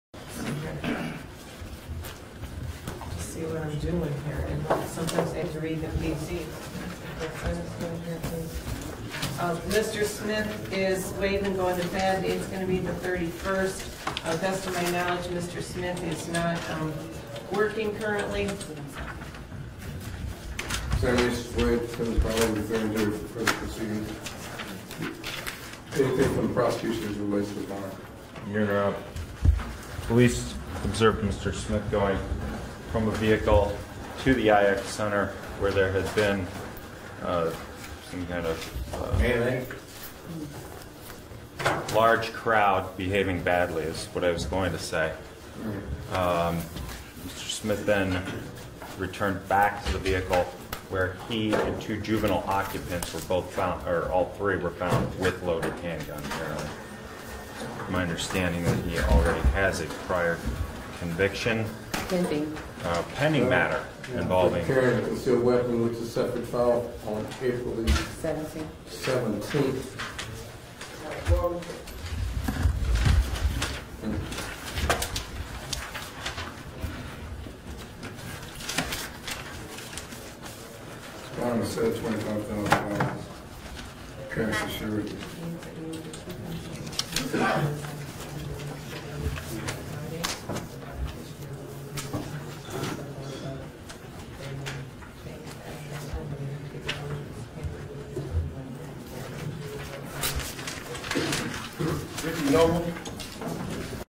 Man pleads not guilty for weapons charge during I-X Center incident
One of the three adults arrested in connection to Saturday's I-X Center incident appeared in court on Monday morning for an arraignment.

Otto Smith is charged with a weapons violation. He pleaded not guilty and is being held on a $25,000 bond.

Cleveland police, with assistance from multiple surrounding police departments, responded to the I-X Center on Saturday night after receiving multiple reports of a shooting, fighting, and flash mobs.

an arraignment for two additional adult suspects is expected to be scheduled on a later date. Two youths were also arrested during the incident and charged with rioting.